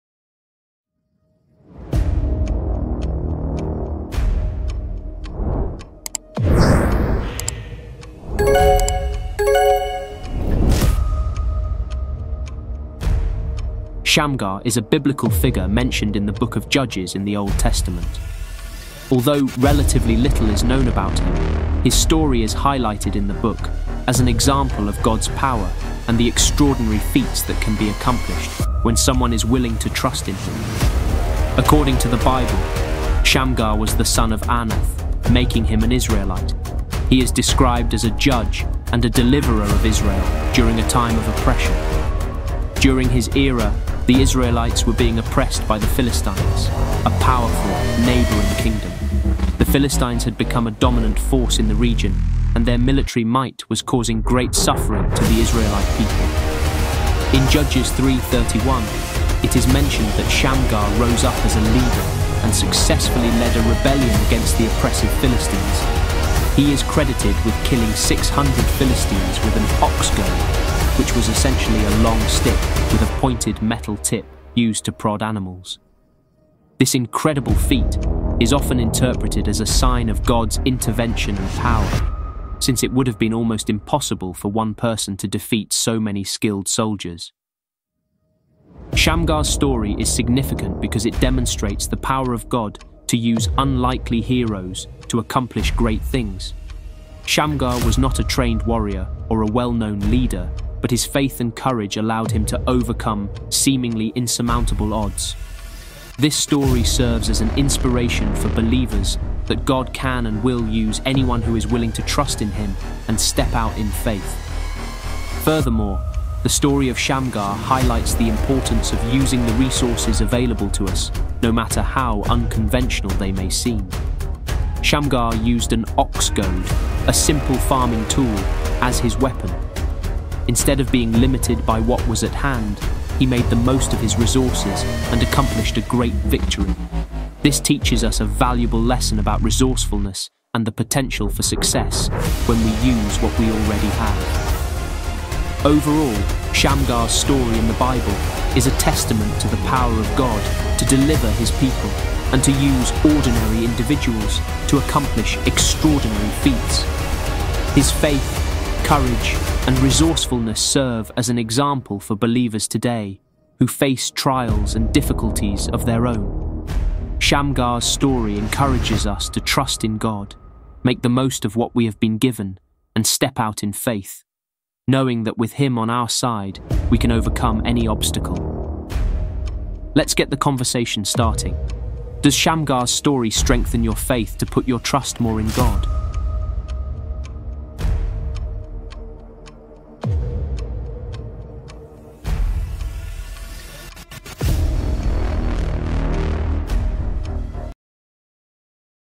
Who Was SHAMGAR ?
Shamgar is a biblical figure mentioned in the book of Judges in the Old Testament. Although relatively little is known about him, his story is highlighted in the book as an example of God's power and the extraordinary feats that can be accomplished when someone is willing to trust in Him.

According to the Bible, Shamgar was the son of Anath, making him an Israelite. He is described as a judge and a deliverer of Israel during a time of oppression. During his era, the Israelites were being oppressed by the Philistines, a powerful neighboring kingdom. The Philistines had become a dominant force in the region and their military might was causing great suffering to the Israelite people.

In Judges 3:31, it is mentioned that Shamgar rose up as a leader and successfully led a rebellion against the oppressive Philistines. He is credited with killing 600 Philistines with an ox goad, which was essentially a long stick with a pointed metal tip used to prod animals. This incredible feat is often interpreted as a sign of God's intervention and power, since it would have been almost impossible for one person to defeat so many skilled soldiers.

Shamgar's story is significant because it demonstrates the power of God to use unlikely heroes to accomplish great things. Shamgar was not a trained warrior or a well-known leader, but his faith and courage allowed him to overcome seemingly insurmountable odds. This story serves as an inspiration for believers that God can and will use anyone who is willing to trust in Him and step out in faith.

Furthermore, the story of Shamgar highlights the importance of using the resources available to us, no matter how unconventional they may seem. Shamgar used an ox goad, a simple farming tool, as his weapon. Instead of being limited by what was at hand, he made the most of his resources and accomplished a great victory. This teaches us a valuable lesson about resourcefulness and the potential for success when we use what we already have.

Overall, Shamgar's story in the Bible is a testament to the power of God to deliver His people and to use ordinary individuals to accomplish extraordinary feats. His faith, courage, and resourcefulness serve as an example for believers today who face trials and difficulties of their own. Shamgar's story encourages us to trust in God, make the most of what we have been given, and step out in faith, knowing that with Him on our side, we can overcome any obstacle.